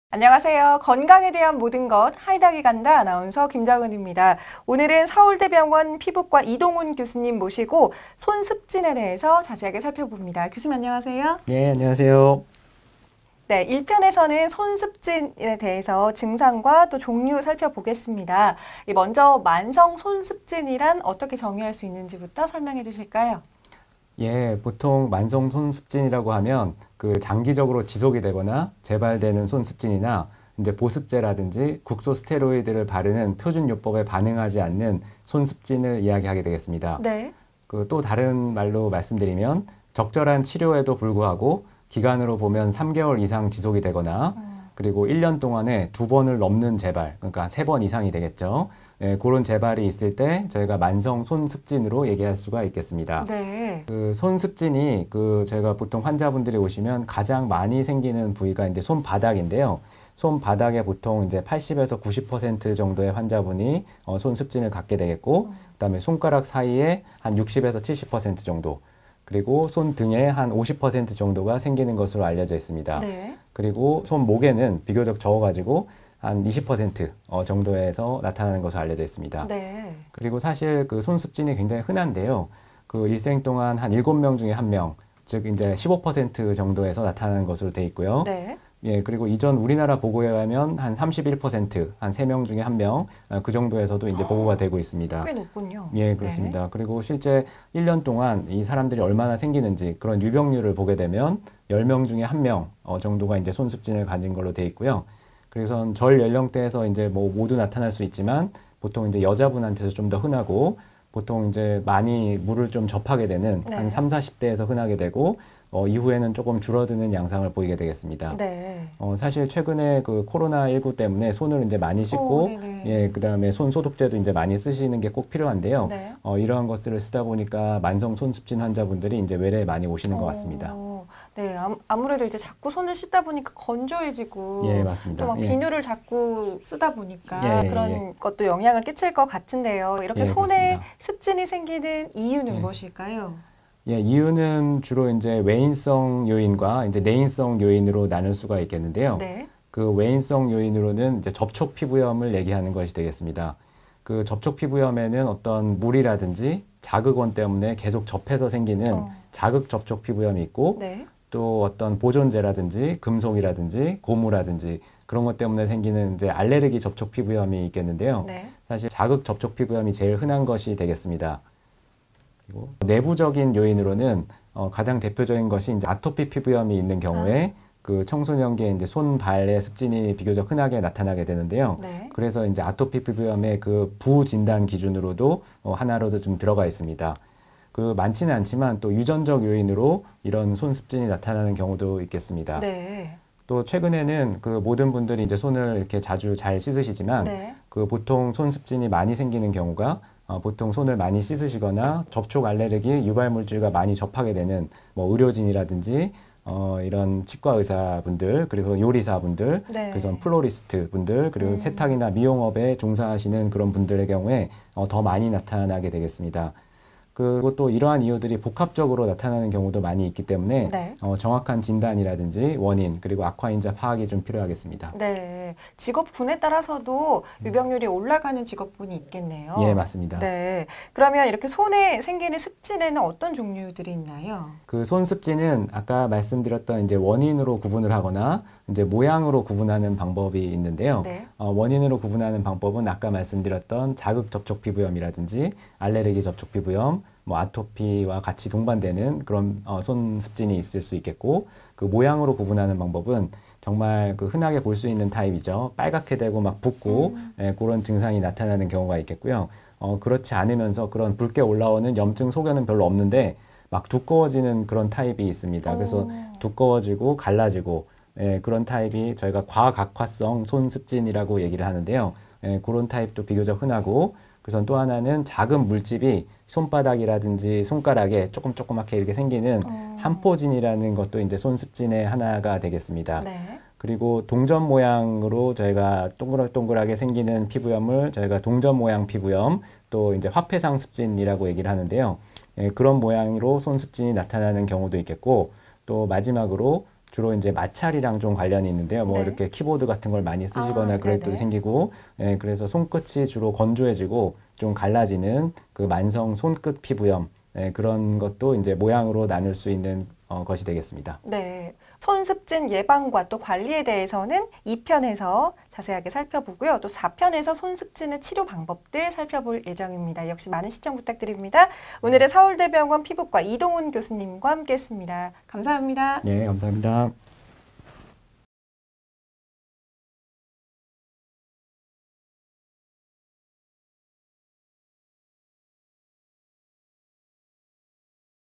손 습진도 종류가 많다고요? 손 습진의 원인과 종류
10명 중 1명에게 생긴다는 손 습진, 만성으로 진단하는 기준은?
손 습진의 원인과 종류에 대해 이동훈 교수님이 알려드립니다!